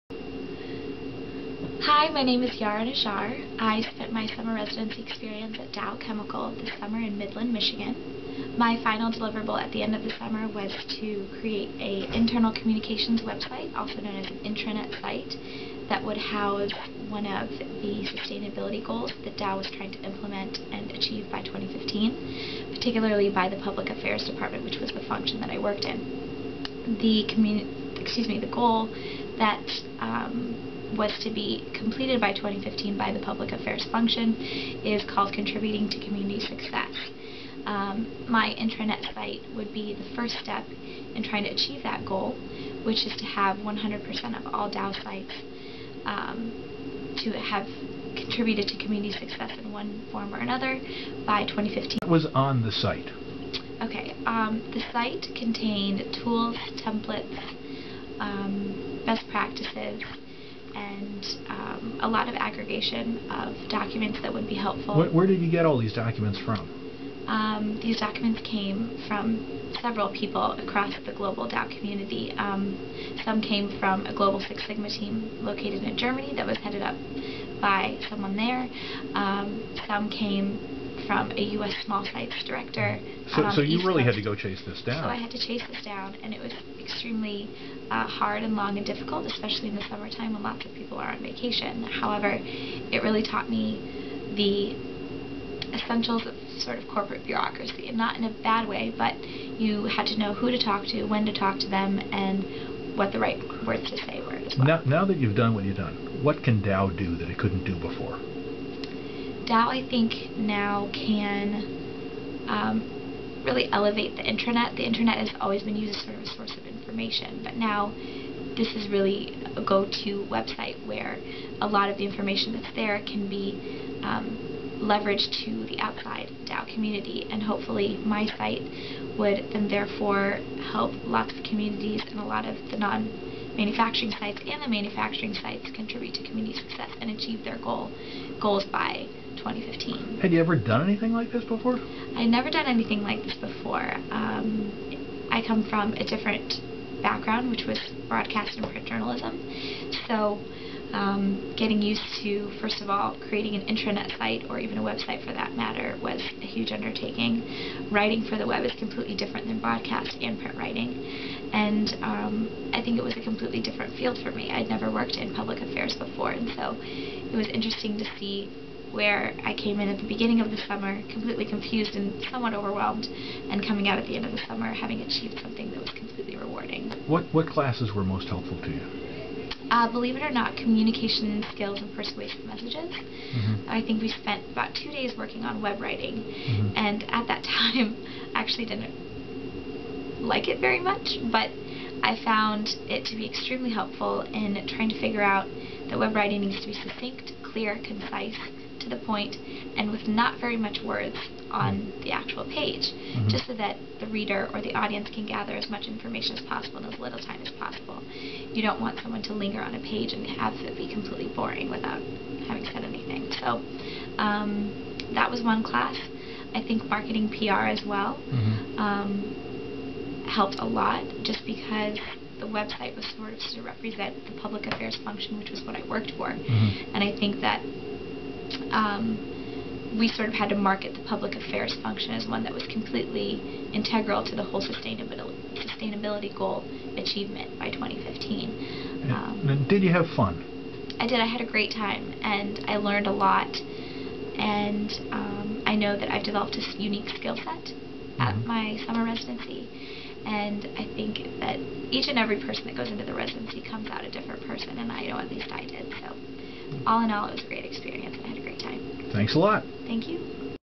Yara Najjar at Dow Chemical
Yara Najjar talks about her IMC Summer Residency project working with Dow Chemical Company in Midland, MI.  Yara worked within the Global Public Affairs and Reputation group.  She developed an intranet site to coordinate Dow's "Sustainability Goals" initiatives.  The site had tools and templates to enable the Public Affairs team to further Community Success at their location.  In her 2nd project, she worked with a global Six Sigma team to create a resource toolkit, and to create a presence for Community Advisory Panels.  She captured best practices, evaluation tools and background information Dow's efforts to achieve Community Success.  She presented her findings to upper level management, and launched her intranet site to Global Public Affairs leaders.